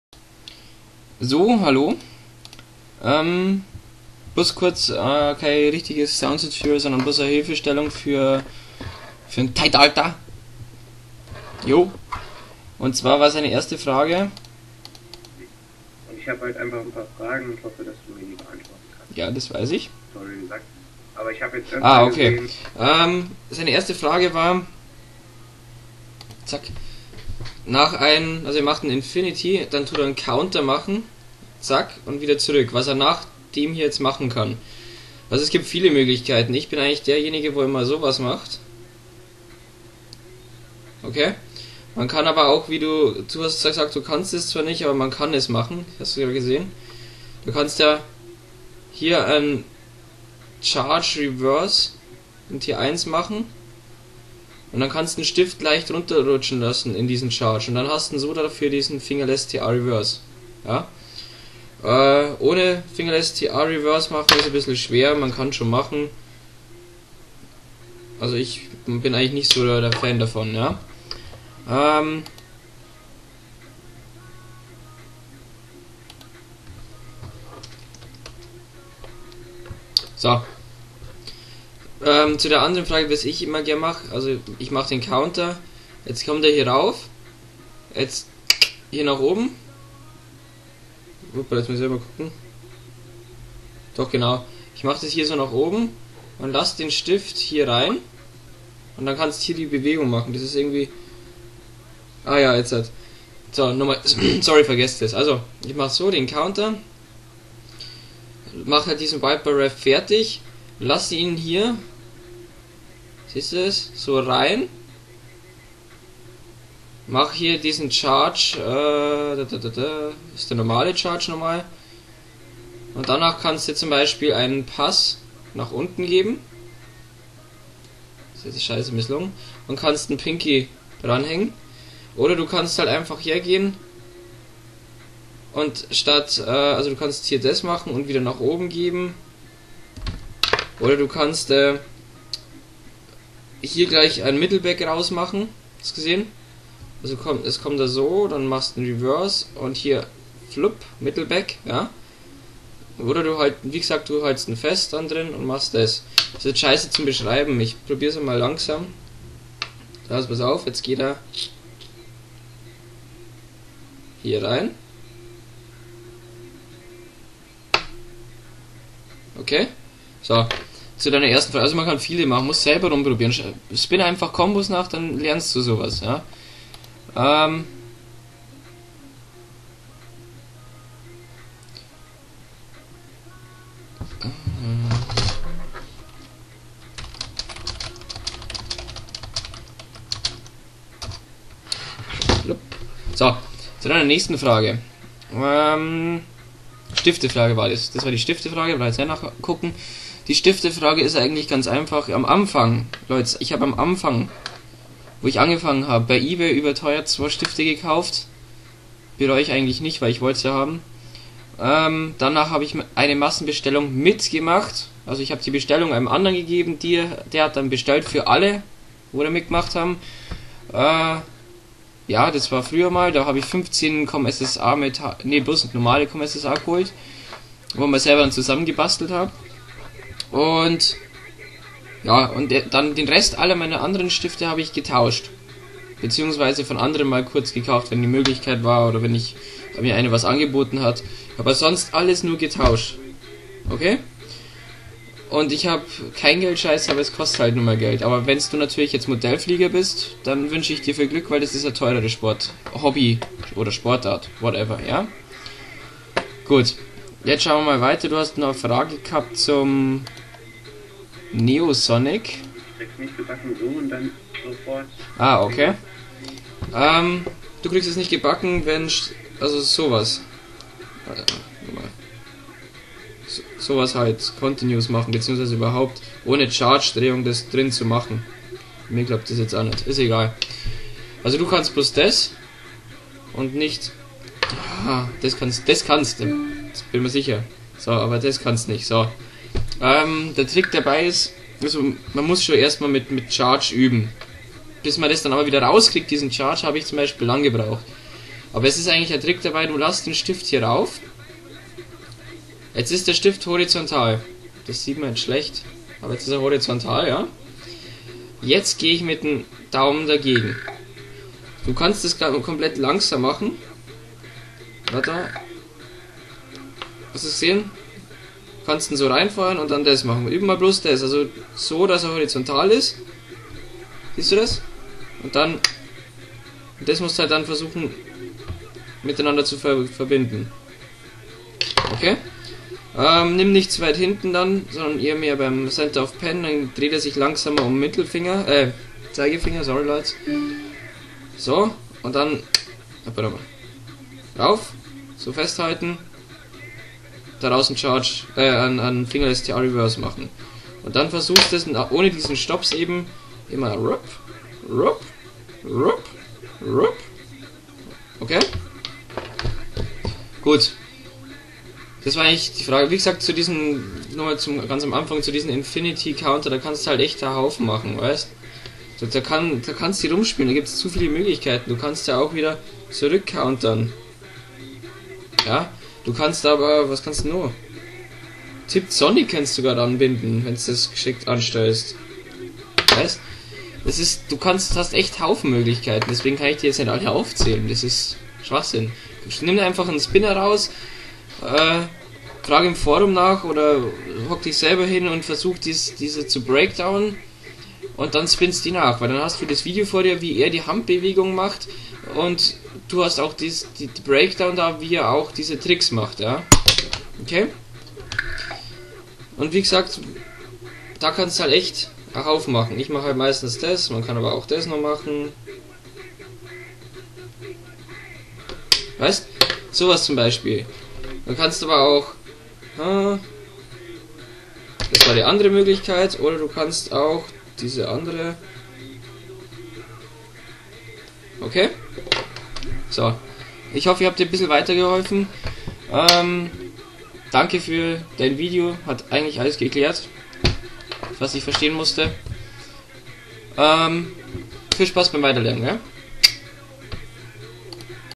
thightalta hilfe, guck in beschreibung was alles
1. was nach wiper rev
2. woher meine ganzen stifte, geldscheisser?
3. neosonic to fl ta rev ohne charge t1 rev zu machen